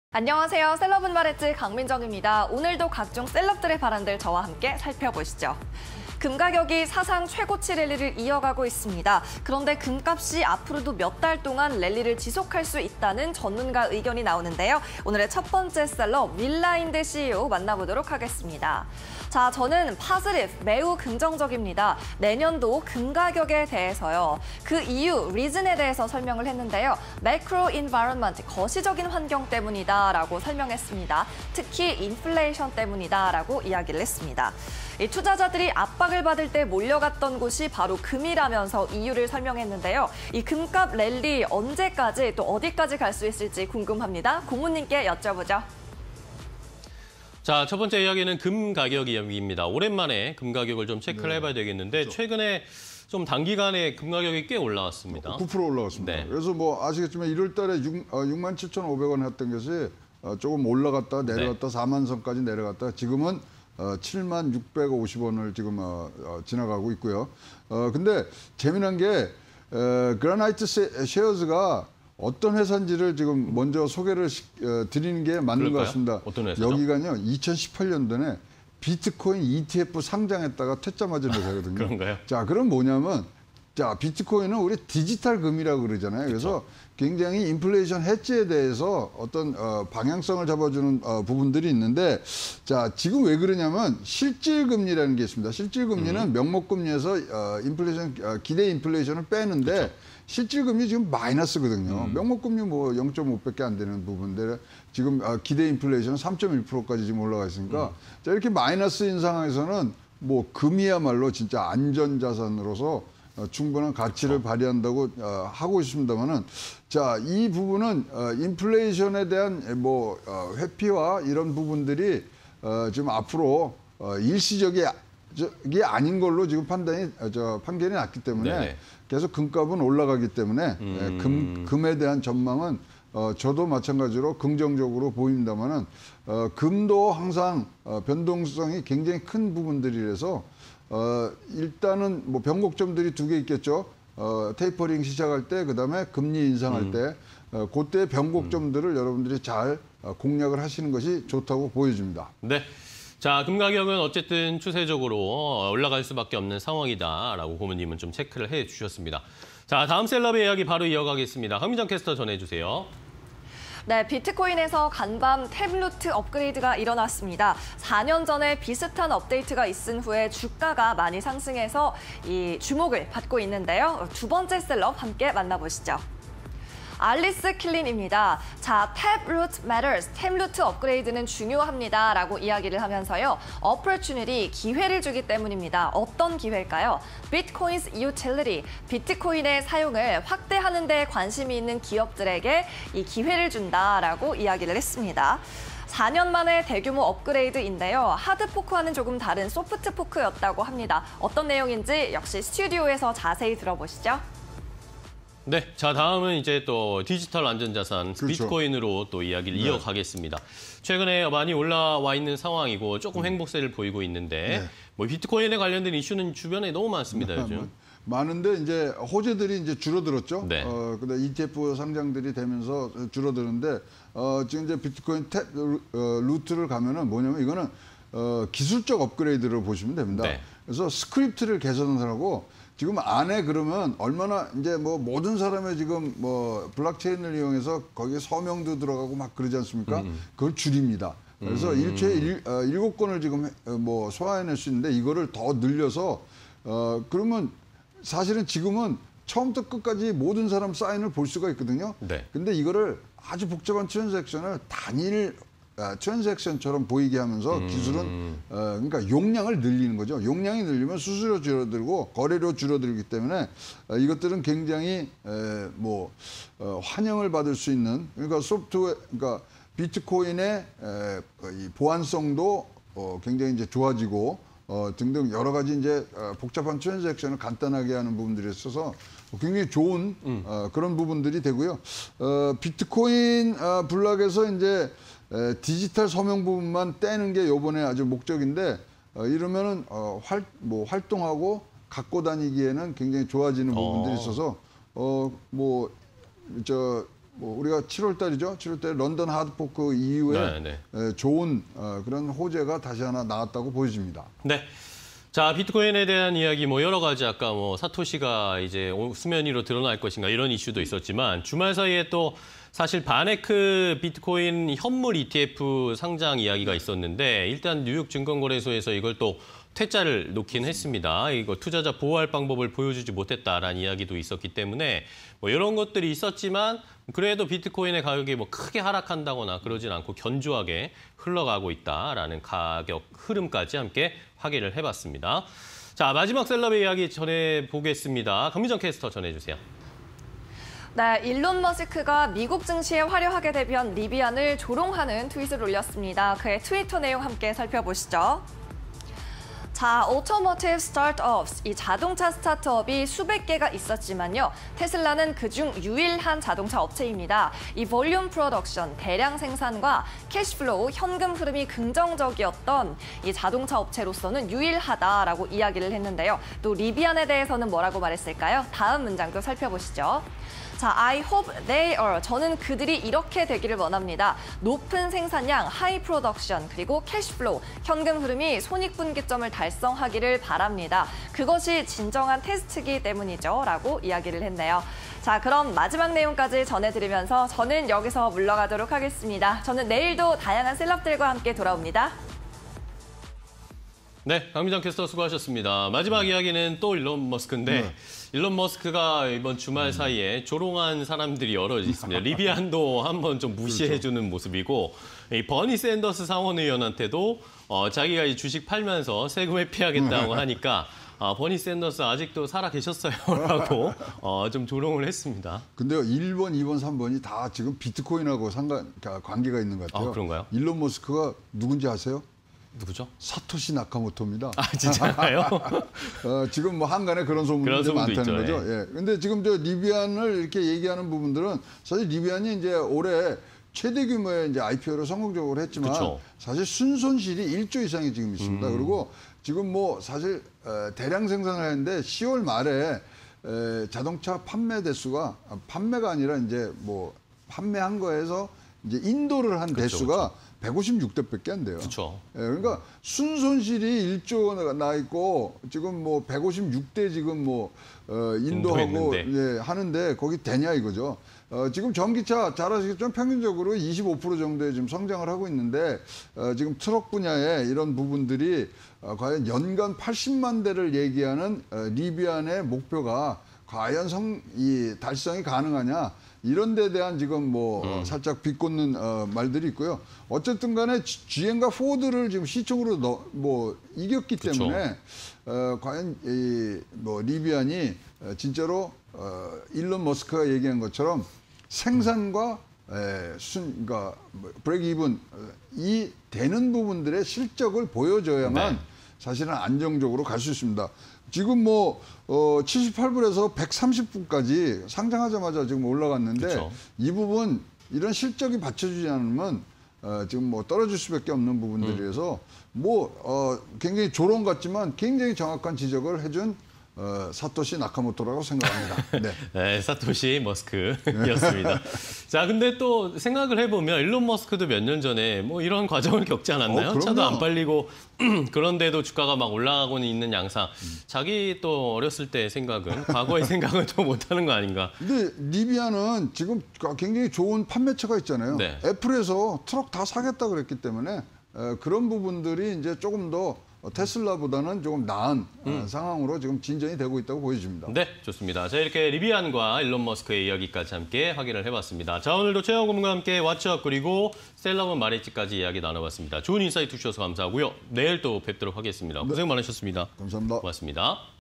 윌 라인드(그라나이트셰어즈 CEO) / 알리스 킬린(스틸마크 설립자) / 일론 머스크(테슬라CEO)_셀럽은 말했지 (20211115)
윌_라인드(그라나이트셰어즈 CEO) / CNBC 인터뷰
"I’m very positive on the outlook for gold for next year and the reason is because of what's going on with the macro environment, particularly inflation"
내년 금 전망에 대해 매우 긍정적이며 그 이유는 거시적 환경, 특히 인플레이션에 대한 상황 때문입니다.

알리스_킬린(스틸마크 설립자) / ​CNBC 인터뷰
"Taproot matters, because it opens a breadth of opportunity for entrepreneurs interested in expanding bitcoin’s utility."
탭루트는 비트코인의 효용 확대에 관심이 있는 기업가들에게 폭넓은 기회를 제공하기 때문에 중요합니다.

일론_머스크(테슬라 CEO) / 트위터
"There have been hundreds of automotive startups, both electric and combustion, but Tesla is only American carmaker to reach high volume production & positive cash flow in past 100 years."
전기차와 내연차를 합해 수백 개의 자동차 스타트업이 있었지만 테슬라는 지난 100년 동안 대량 생산과 긍정적인 현금 흐름에 도달한 유일한 미국 자동차 제조업체입니다.

"I hope they’re able to achieve high production and breakeven cash flow. That is the true test."
그들(리비안)이 높은 생산량과 손익분기점 현금 흐름을 달성할 수 있기를 바랍니다. 그것이 진정한 시험이죠.